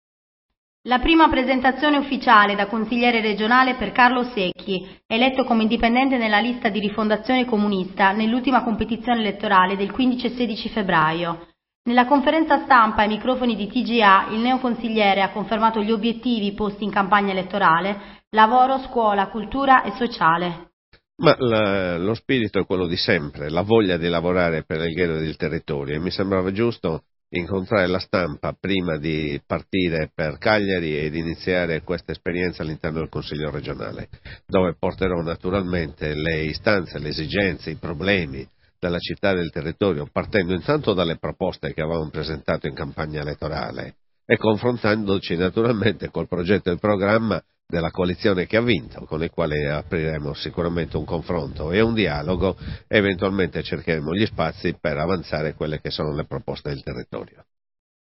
«A Cagliari i problemi di Alghero»
A pochi giorni dall´insediamento a Cagliari, Carlo Sechi, eletto come indipendente nella lista di Rifondazione Comunista, ha presentato ieri la prima conferenza stampa da consigliere regionale.